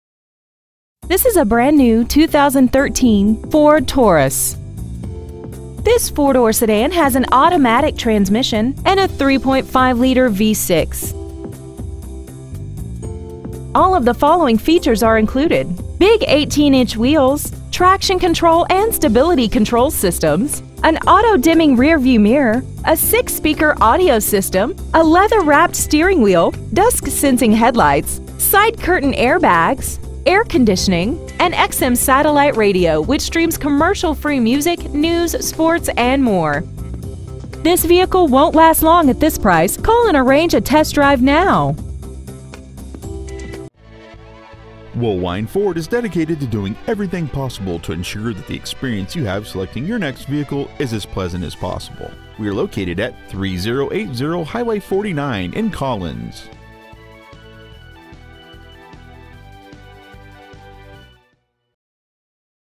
2013 Ford Taurus Collins MS
We have been honored to serve the Collins MS area , we promise that your experience at our dealership will exceed your expectations !

 Year : 2013
 Make : Ford
 Model : Taurus
 Engine : 3.5L V6 24V MPFI DOHC FLEXIBLE FUEL
 Trans . : 6-SPEED AUTOMATIC
 Exterior : RUBY RED METALLIC TINTED CLEARCOAT

 Interior : DUNE
 Stock : 3554